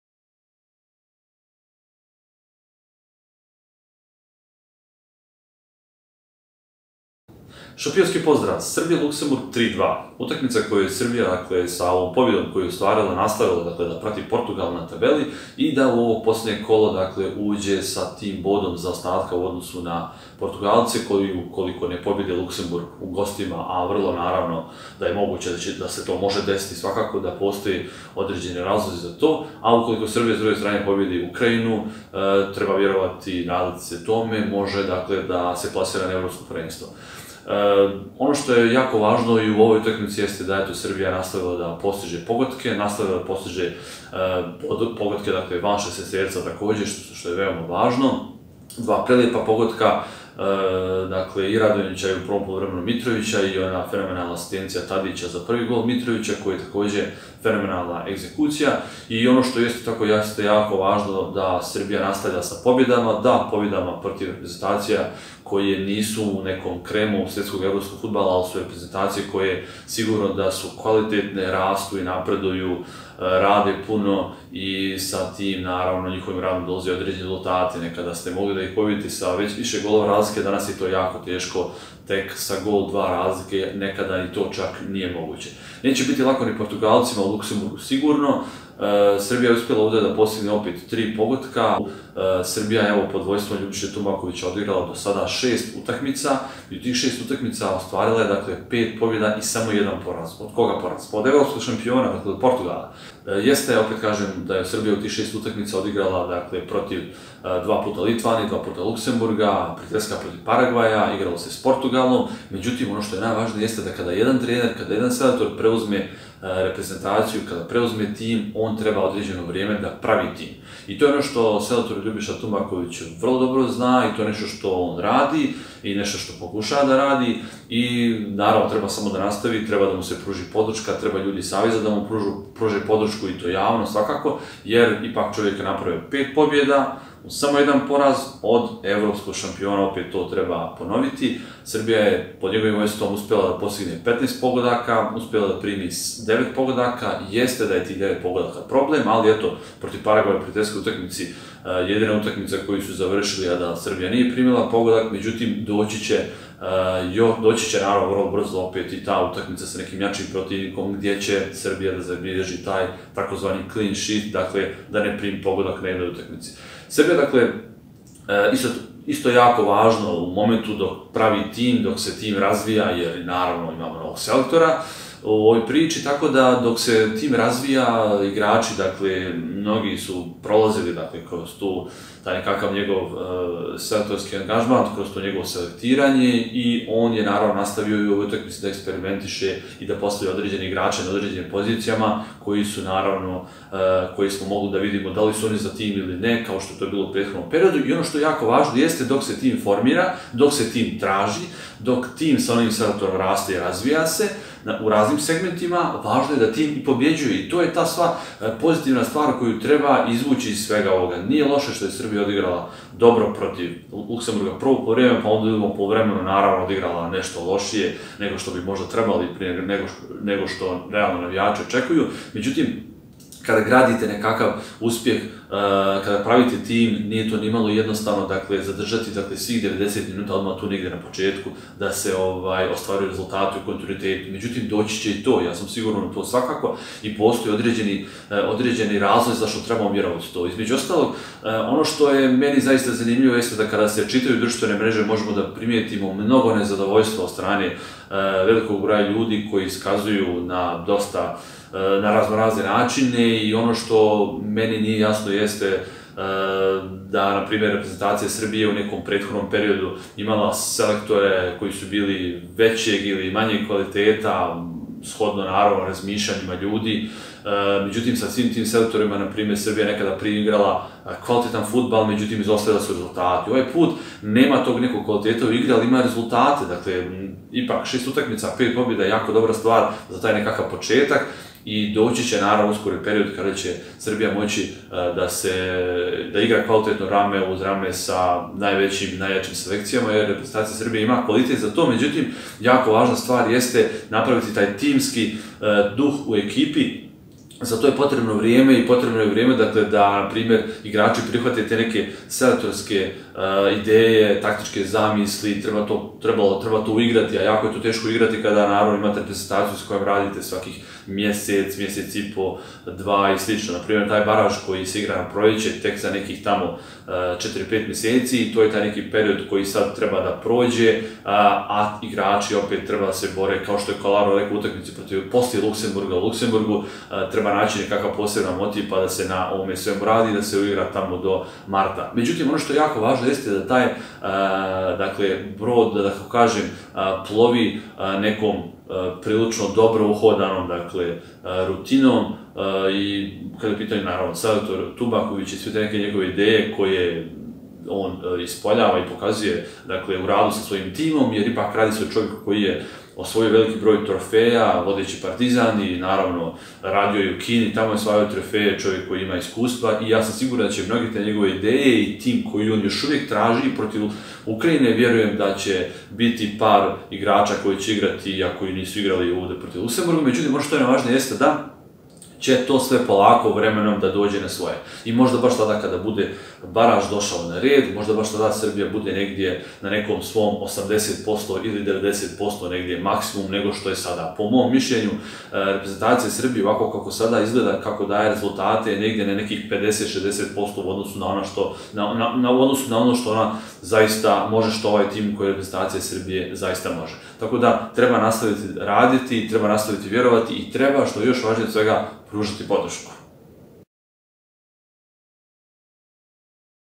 Kako Srbija napreduje sa selektorom Tumbakovićem? - Podrška ili ne?
Kako Srbija napreduje sa selektorom Tumbakovićem? Da li postoje realni razlozi za podršku ili ipak ne? Kakav bilans ima kao selektor u prvih 6 utakmica sa reprezentacijom Srbije? Koje rotacije je pravio u utakmici sa Luksemburgom?